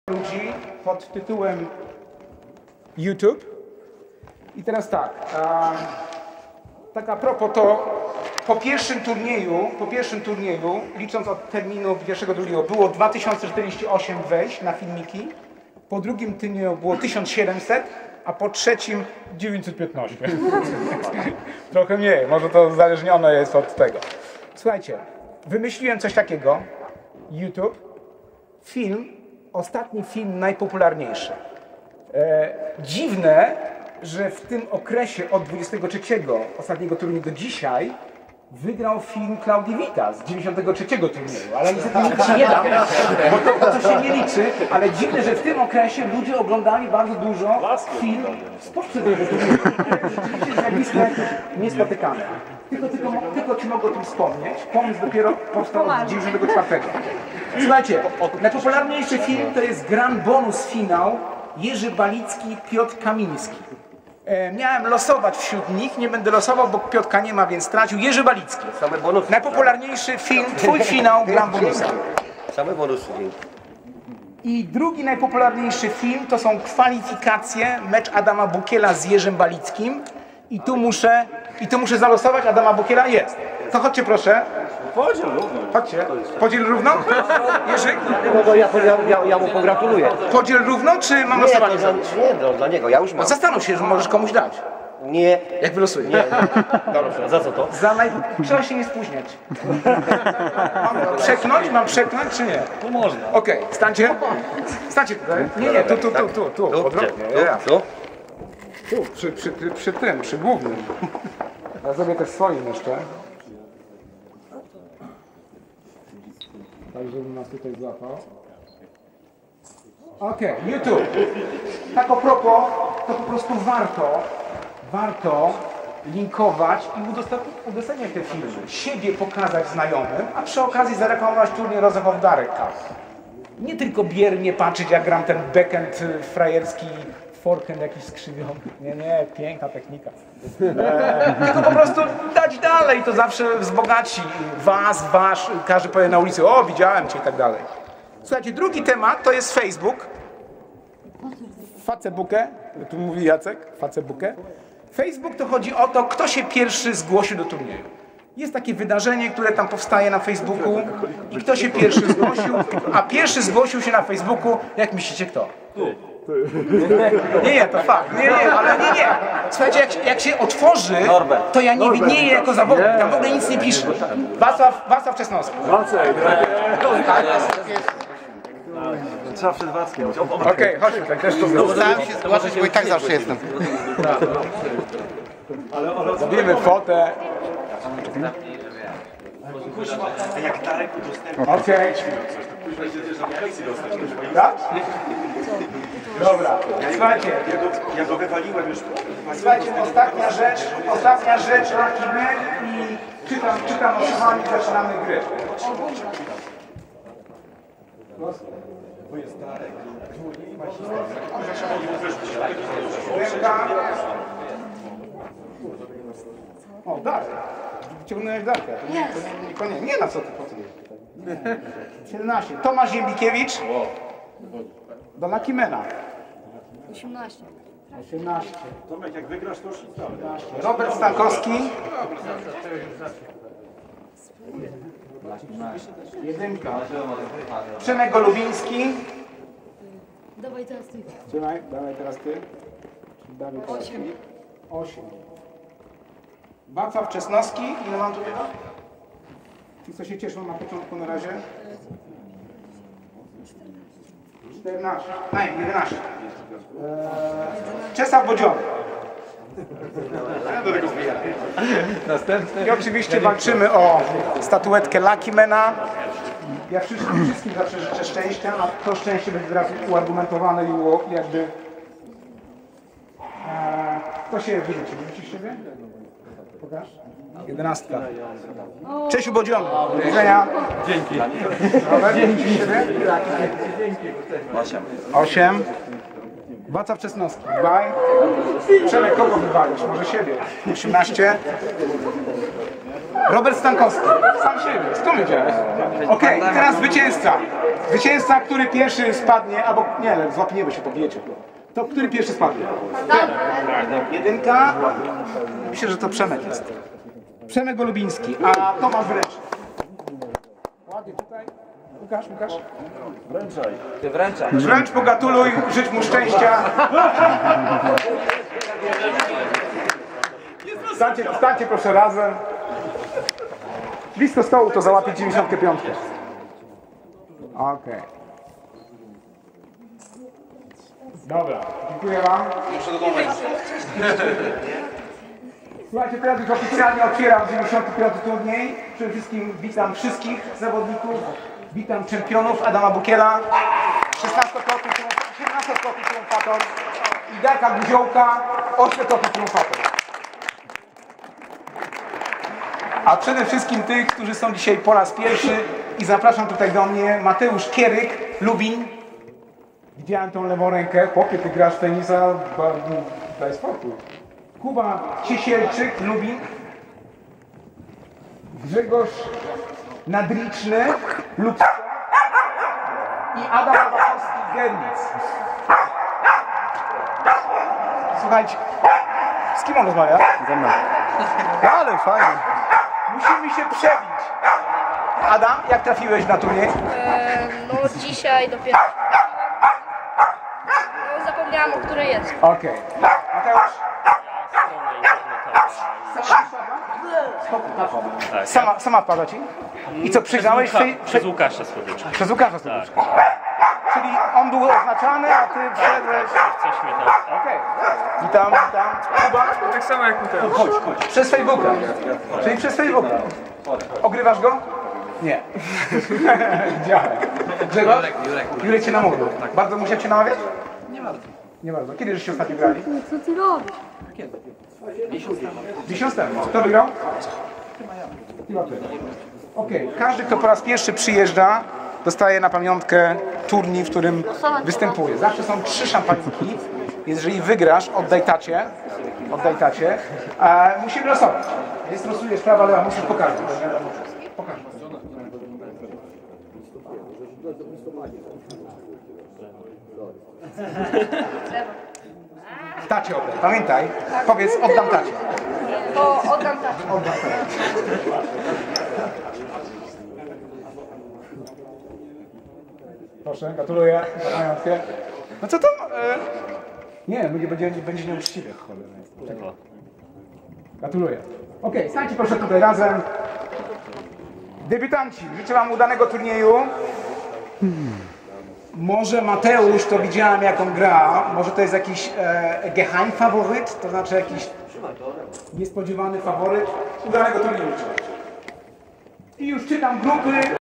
95 ROSENHOF darek CUP. Awards & Newcomers & Lucky Men.
95 International Tennis Tournament ROSENHOF darek CUP, Görlitz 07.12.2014 - YouTube and Facebook Awards & Newcomers   ***   Nagrody specjalne oraz powitanie Debiutantów.